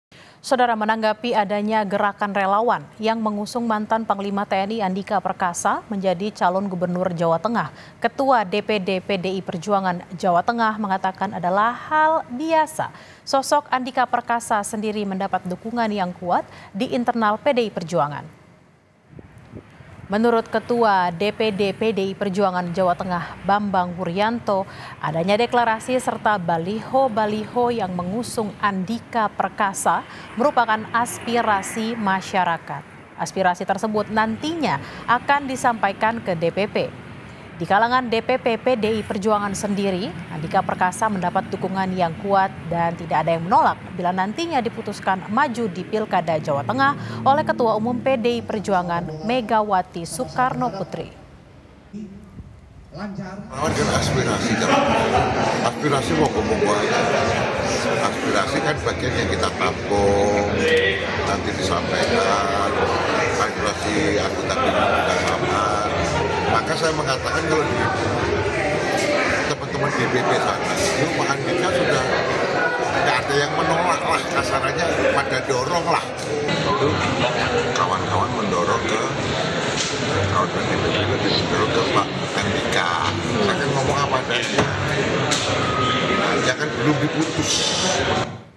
Dukungan Andika Perkasa Maju di Pilgub Jateng
KOMPAS.TV - Baliho Dukung Andika Perkasa Maju Pilgub Bermunculan di Semarang, Ini Kata PDIP